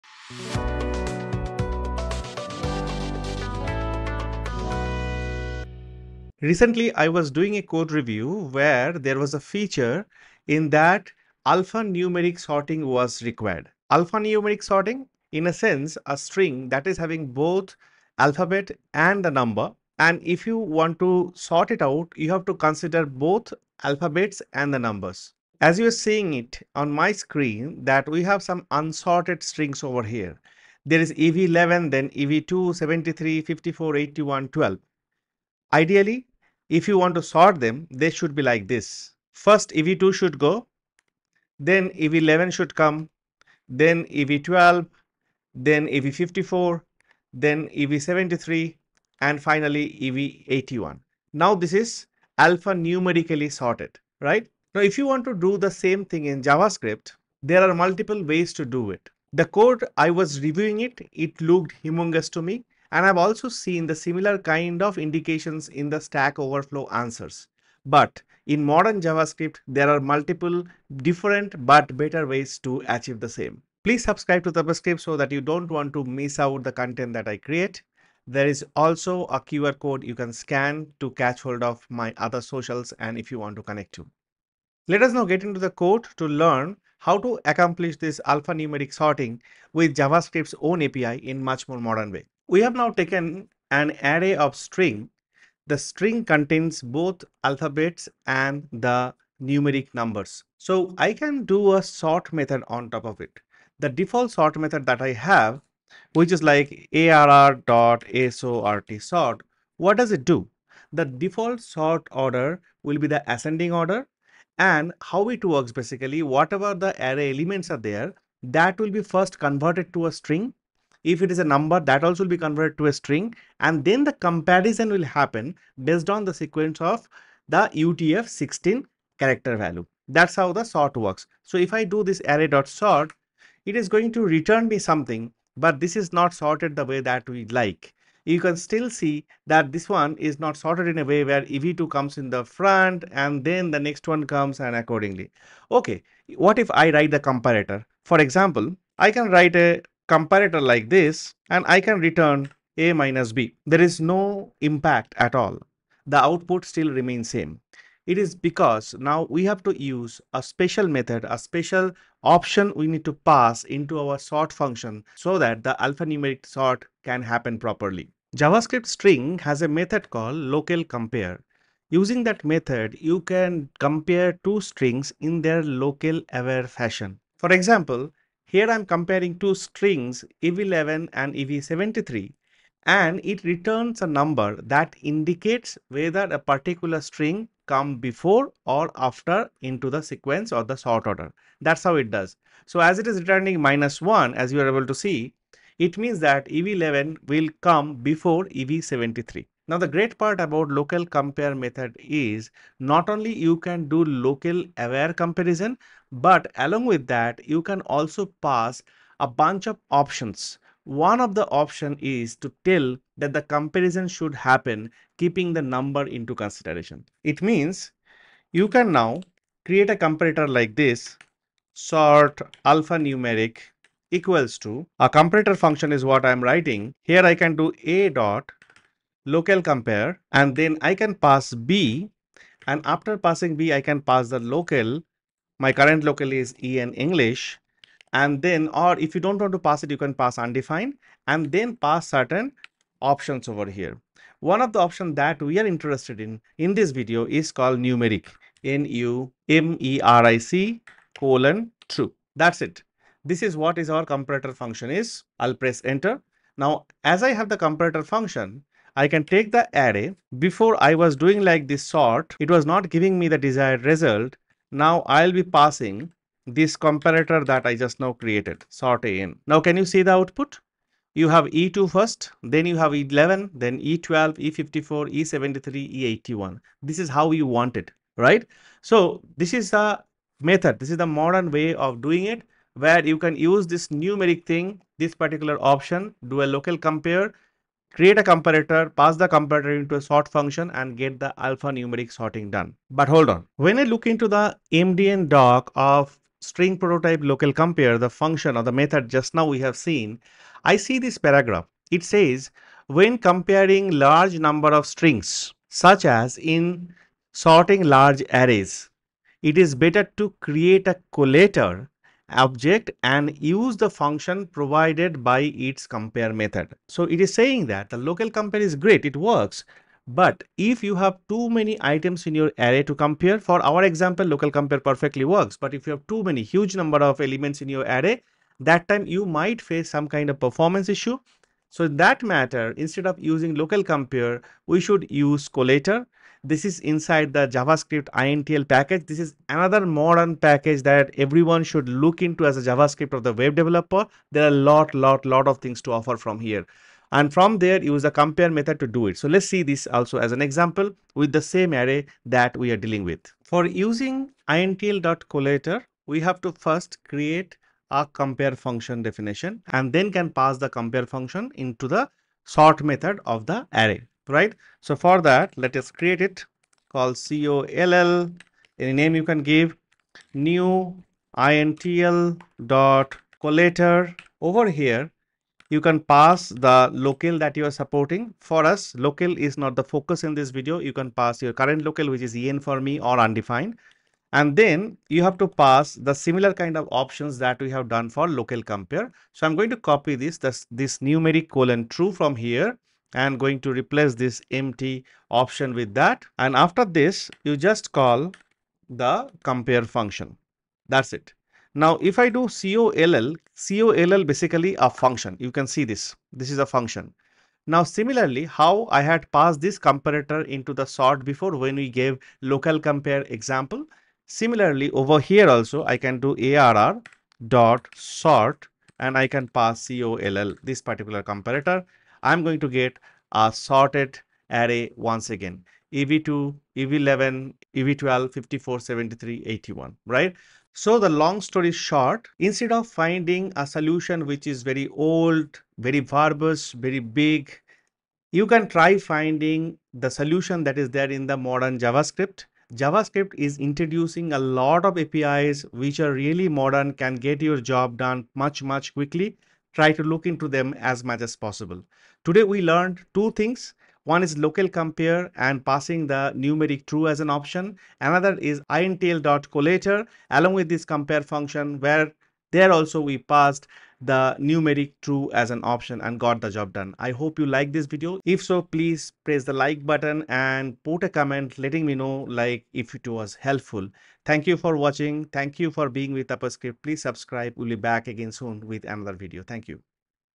How To Sort An Alphanumeric String Array In JavaScript?
JavaScript is changing rapidly with the inclusion of modern APIs that help in the easiest way of handling things. In this video, we will discuss one such methodology to handle Alphanumeric string sorting.

Timecodes
0:00 - The Problem
00:26 - What is Alphanumeric Sorting
01:25 - Array Sorting
02:48 - The Modern Method 1
04:53 - Performance
05:54 - The Modern Method 2
07:11 - The Modern JavaScript